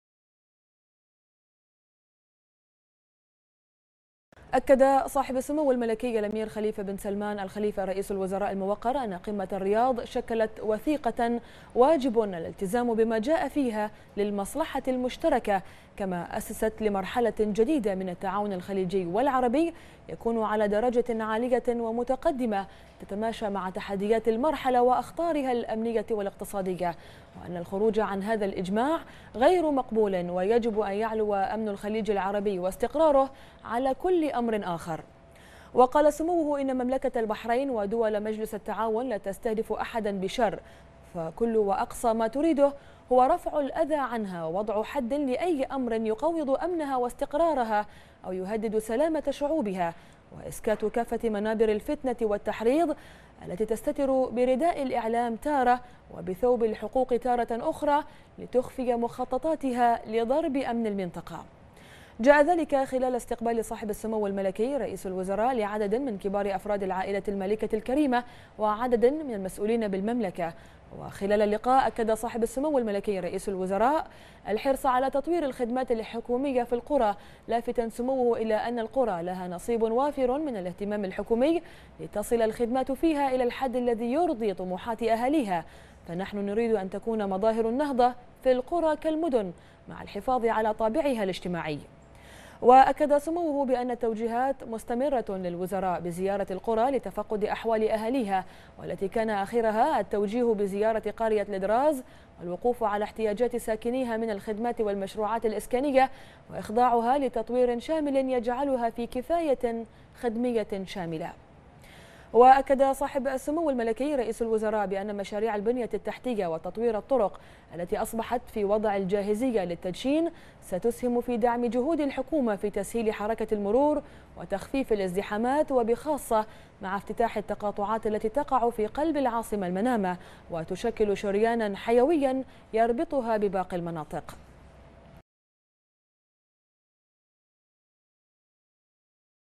البحرين: سمو رئيس الوزراء يستقبل عدد من كبار أفراد العائلة المالكة والمسؤولين بالمملكة
المنامة في 6 يونيو / بنا / أكد صاحب السمو الملكي الأمير خليفة بن سلمان آل خليفة رئيس الوزراء الموقر أن قمة الرياض شكلت وثيقة واجب الالتزام بما جاء فيها للمصلحة المشتركة، كما أسست لمرحلة جديدة من التعاون الخليجي والعربي يكون على درجة عالية ومتقدمة تتماشى مع تحديات المرحلة وأخطارها الأمنية والاقتصادية، وان الخروج عن هذا الاجماع غير مقبول ويجب أن يعلو أمن الخليج العربي واستقراره على كل أمر آخر، وقال سموه "إن مملكة البحرين ودول مجلس التعاون لا تستهدف أحدا بشرٍ فكل وأقصى ما تريده هو رفع الأذى عنها ووضع حد لأي أمر يُقوض أمنها واستقرارها أو يُهدد سلامة شعوبها، واسكات كافة منابر الفتنة والتحريض التي تستتر برداء الاعلام تارة وبثوب الحقوق تارة أخرى لتخفي مخططاتها لضرب أمن المنطقة".